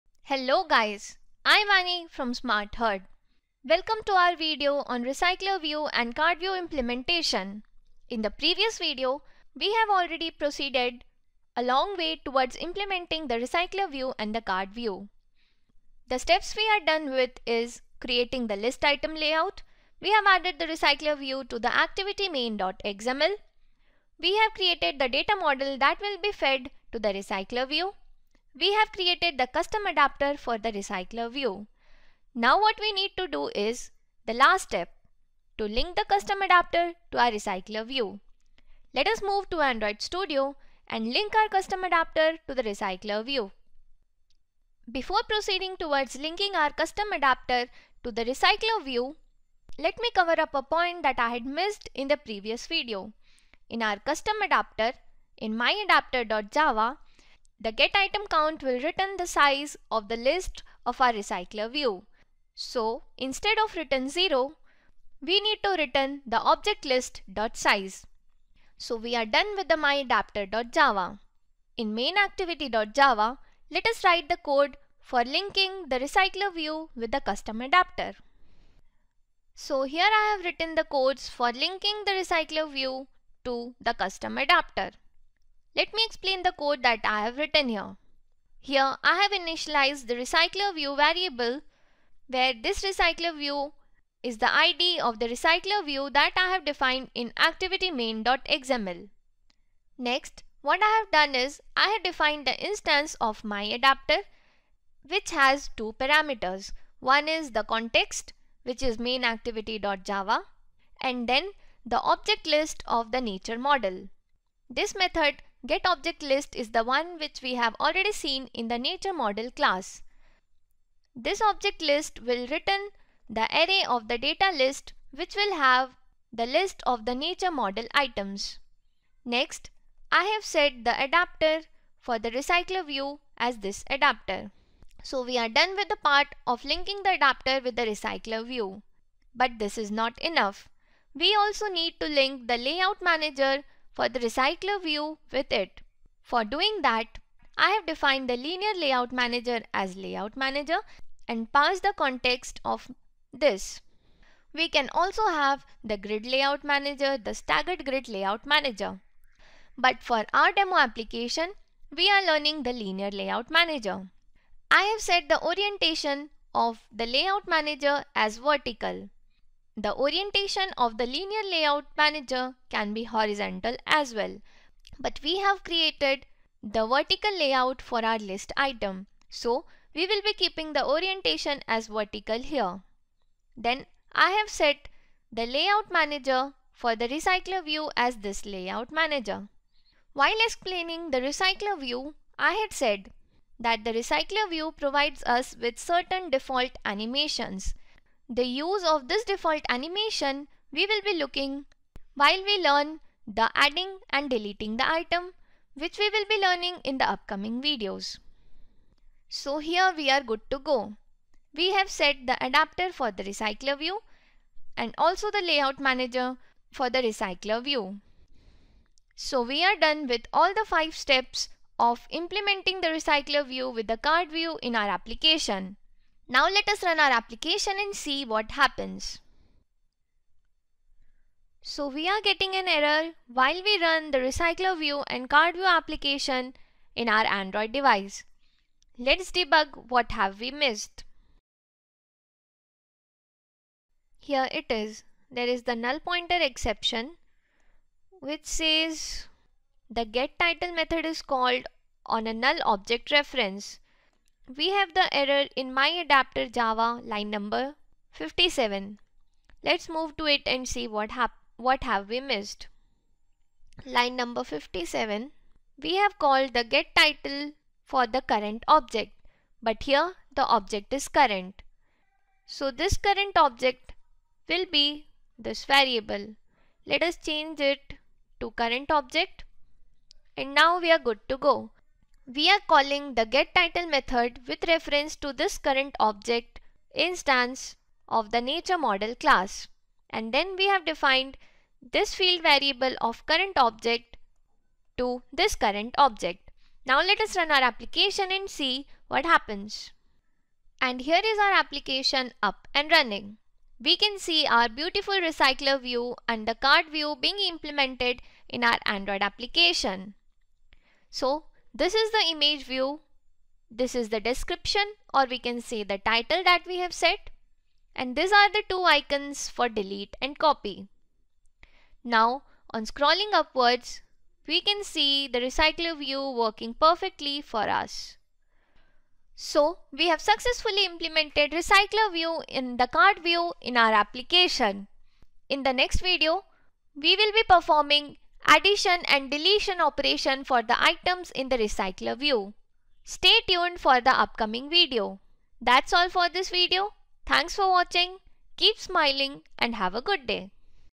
#9.3 Android Recycler View with Card View example. Material Design | Flexible ListView
Implement RecyclerView and CardView in Android. Create a complete demo from scratch. Each method of Recycler Adapter such as onBindView, onCreateViewHolder and ViewHolder class is well explained in detail.